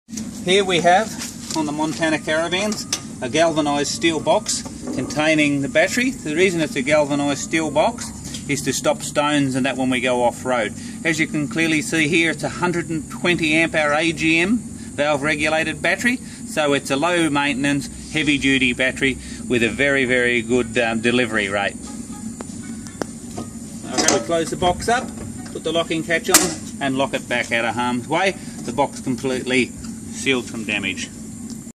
Montana Caravan: Galvanised Battery Box
The Montana has an excellent battery which is housed in a stone proof galvanised box